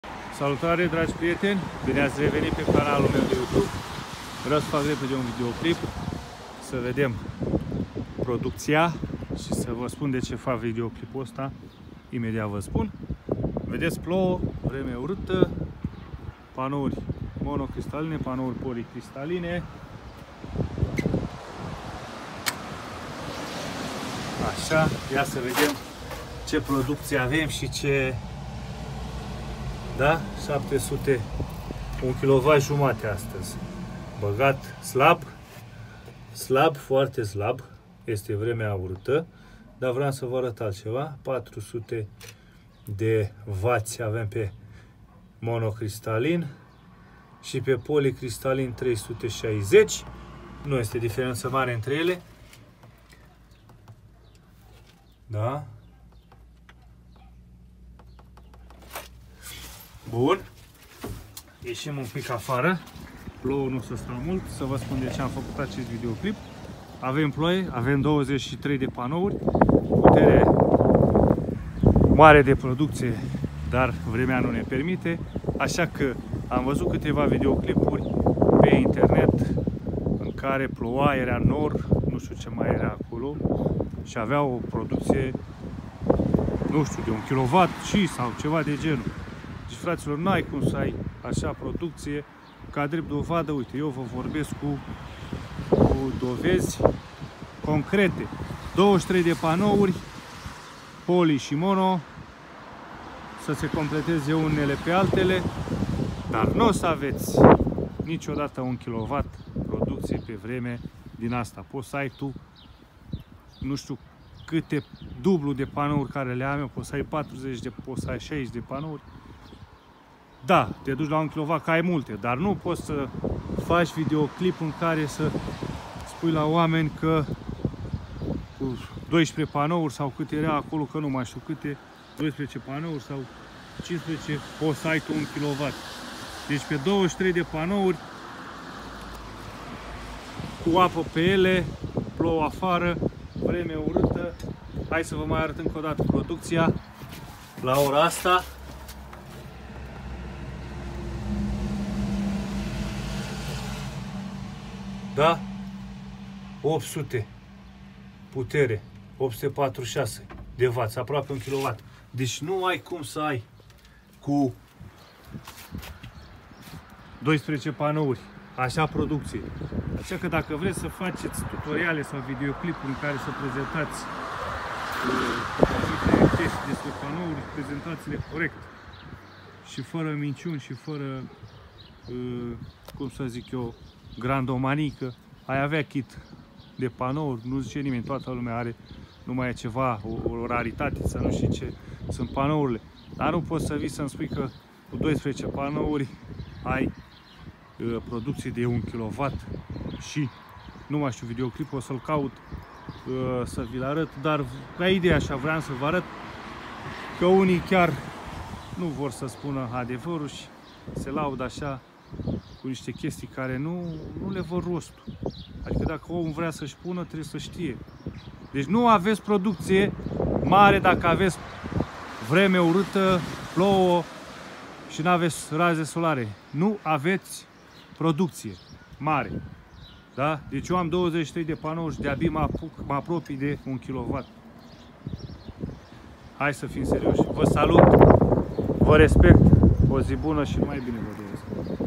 Panouri fotovoltaice - ATENTIE DACA ESTE NOR SI URAT AFARA - nu ai productie mai ales pe 12 panouri!
Panouri fotovoltaice - ATENTIE DACA ESTE NOR SI URAT AFARA - nu ai productie mai ales pe 12 panouri! Nu mai spuneti prostii ca unii oameni chiar cred ca asa este !!! Fiti corecti !!